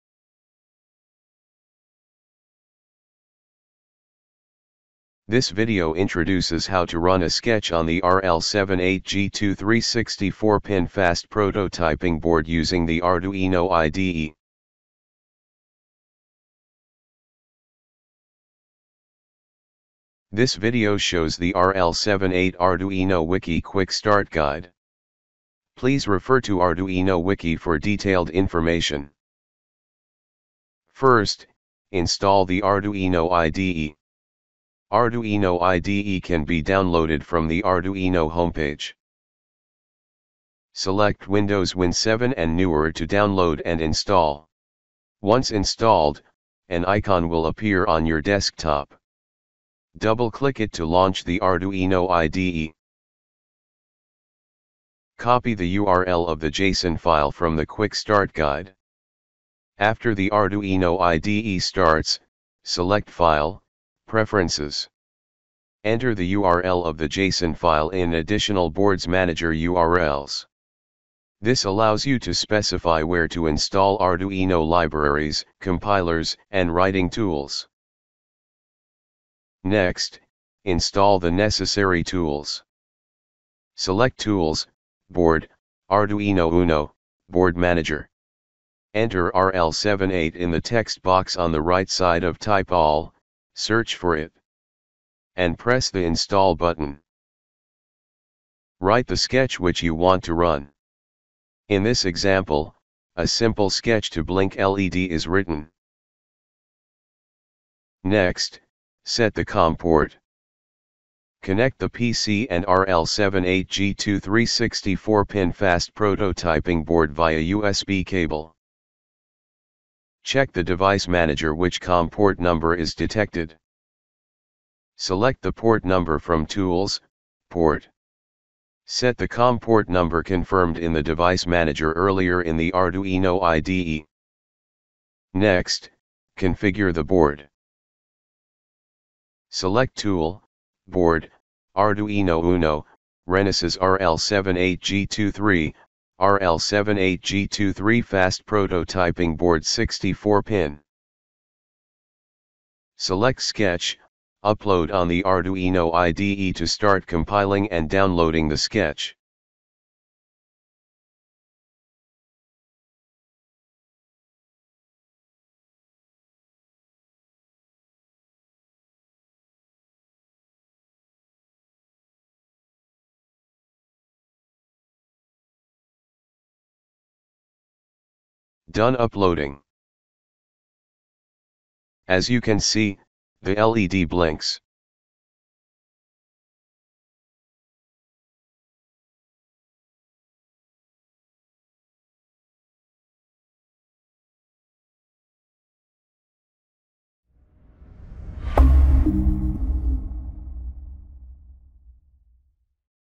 RL78 Arduino Quick Start Guide
This video introduces how to run a sketch on the RL78/G23 Fast Prototyping Board using the Arduino IDE.
Related resources: Quick Start Guide page (GitHub)

00:00 Opening
00:30 Install the Arduino IDE
00:55 Preferences setting
01:19 Install the tools
01:39 Write the sketch
01:48 COM port setting
02:15 Board setting
02:33 Compile, upload, LED blinking